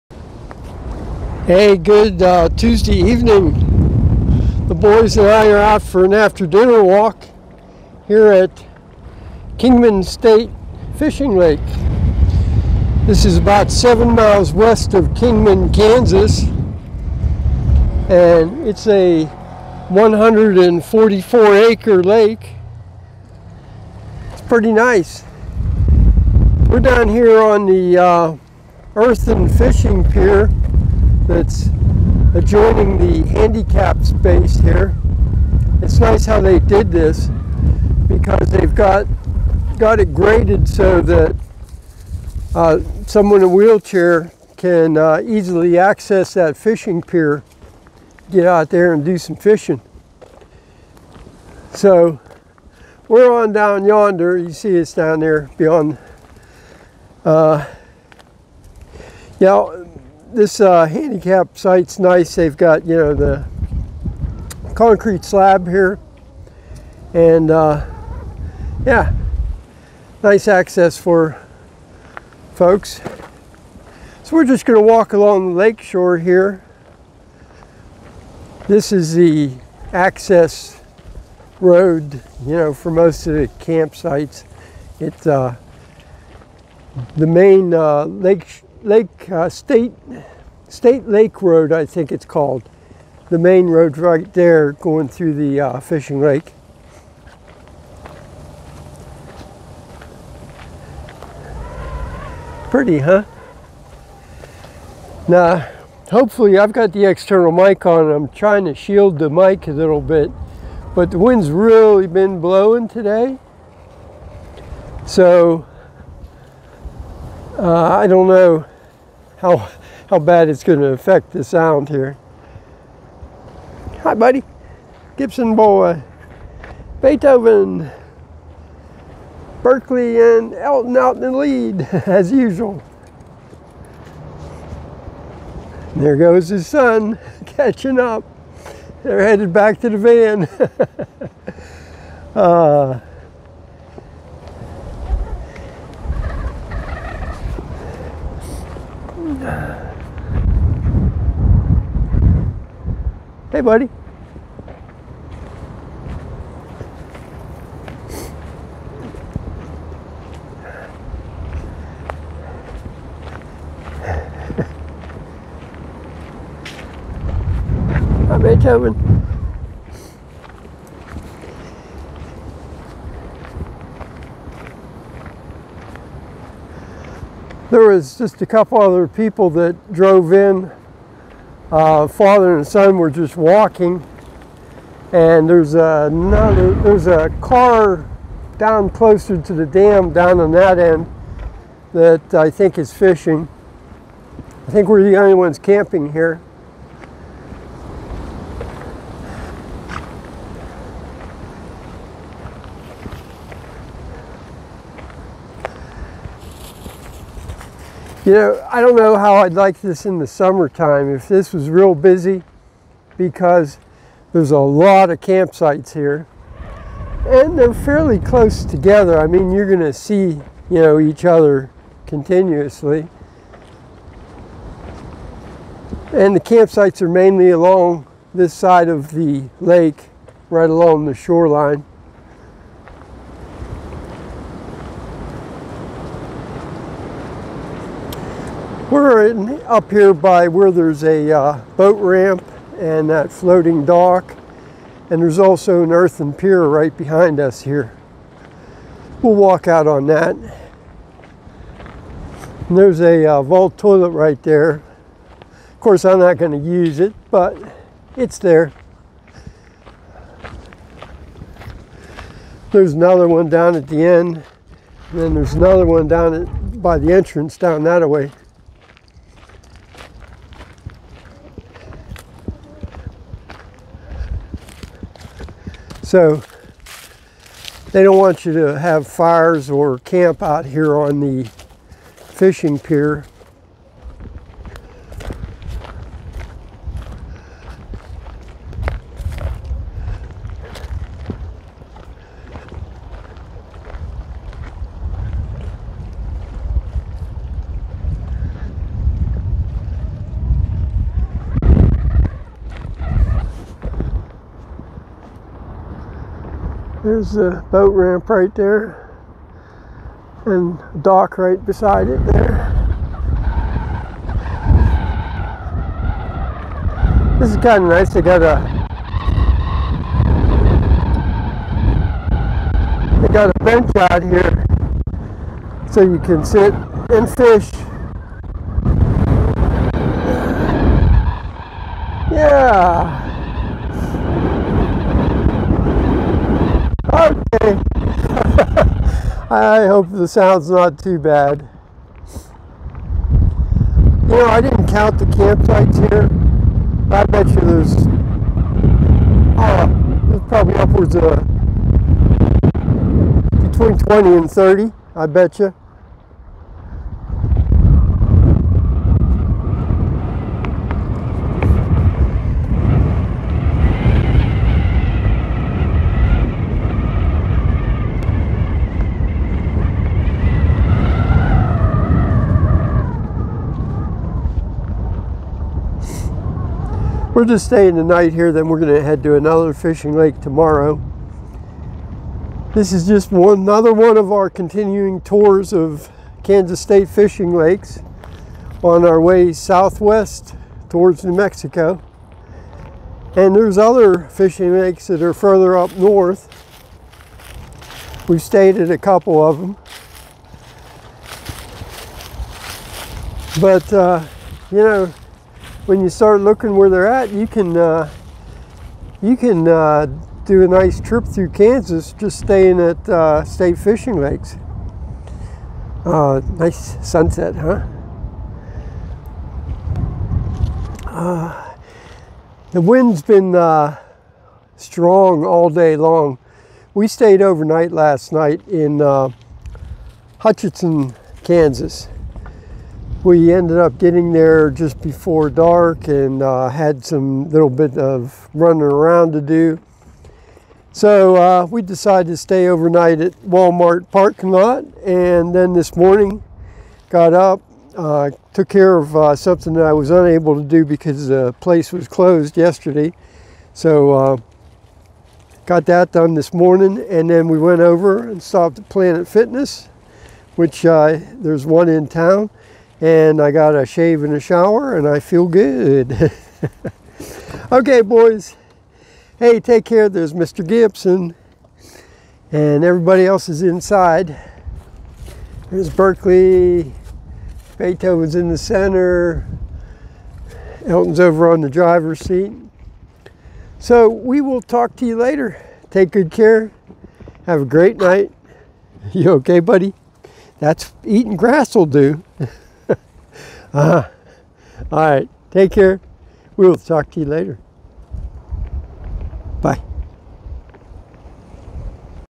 11-23-2021, Walkin' & Talkin' in the Wind at Kingman State Fishing Lake
The Boys and I are taking an after-dinner walk along the lake shore at Kingman State Fishing Lake near Kingman, Kansas. The strong wind caused noise and distortions in the sound.